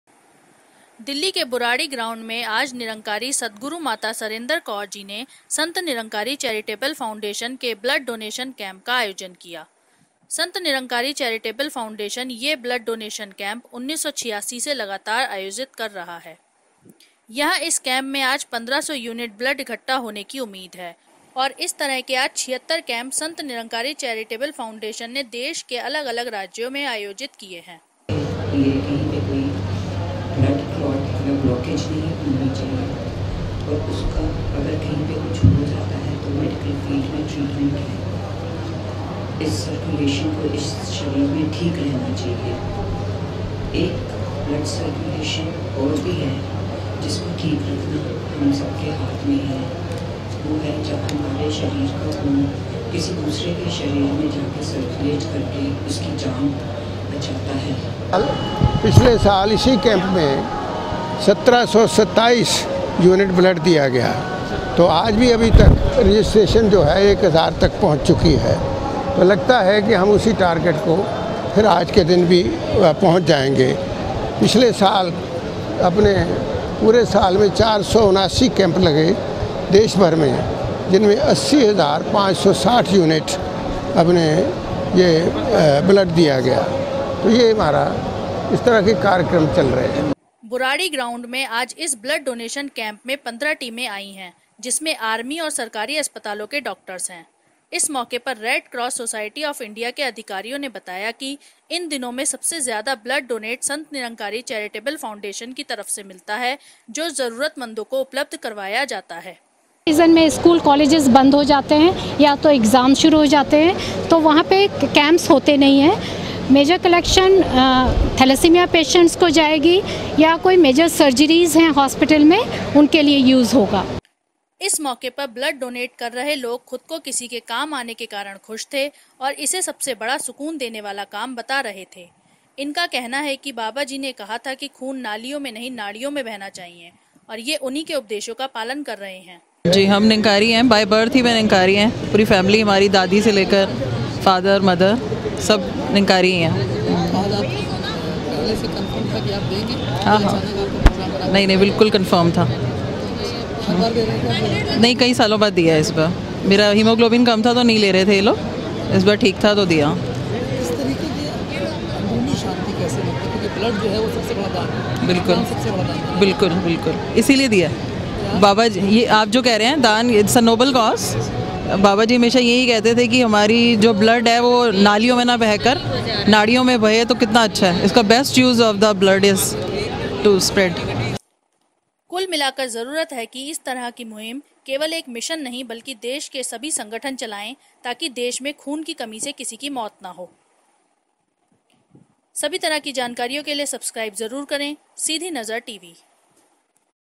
संत निरंकारी चैरिटेबल फाउनडेशन द्धारा लगाया गया रक्तदान शिविर || Sidhi Nazar
वी  ओ ---

दिल्ली के बुराड़ी ग्राउंड में आज निरंकारी माता जी ने संत निरंकारी चैरिटेबल फाउनडेशन के ब्लड डोनेशन कैम्प का शुभारम्भ किया .. संत निरंकारी चैरिटेबल फाउनडेशन ये ब्लड डोनेशन कैम्प 1986 से लगातार आयोजित कर रहा है .. यहा इस कैम्प में आज 1500 यूनिट ब्लड इक्कठा होने की उम्मीद है और इस तरह के आज 76 कैम्प संत निरंकारी चैरिटेबल फाउनडेशन ने देश के अलग अलग राज्यों में आयोजित किये है . ..

बाईट -- कृपासागर मीडिया कोंरडीनेटर संत निरंकारी चैरिटेबल फाउनडेशन

बाईट -- नाम और पद बोला है   ..

वी ओ --2

आज बुराड़ी ग्राउंड में आज इस शुभारम्भ में पन्द्रह टीमे आई है जिनमें आर्मी और सरकारी अस्पतालों के डोक्टरस है   .. इस मौके पर रेड क्रॉस सोसाइटी ऑफ़ इंडिया के अधिकारियो ने बताया की इन दिनों में सबसे ज्याद ब्लड डोनेट संत निरंकारी चैरिटेबल फाउनडेशन की तरफ से मिलता है जो जरूरतमन्दो को उपलब्ध करवाया जाता है   ..

बाईट ---  डायरेक्टर ब्लड बैंक रेड क्रॉस सोसाइटी नेशनल हैड क्वार्टर्स

वी  ओ -- 3

इस मौक पर ब्लड डोनेट कर रहे लोग खुद को किसी के काम आने के कारण खुश थे और इसे सबसे बड़ा सुकून देने वाला बता रहे थे   .. और इनका कहना है की बाबा जी ने कहा था की खून नालियों में नहीं नाडियों में बहना चाहिए ...और ये उन्ही के उपदेशों का पालन कर रहे है ...

बाईट -- महिला ब्लड डोनर

वी  ओ --- 4

कुलमिलाकर जरूरत है इस तरह की मुहीम ये एक मिशन नही बल्कि देश के सभी संगठन चलाए ताकिदेश में ब्लड की कमी से किसी कीमौत नहो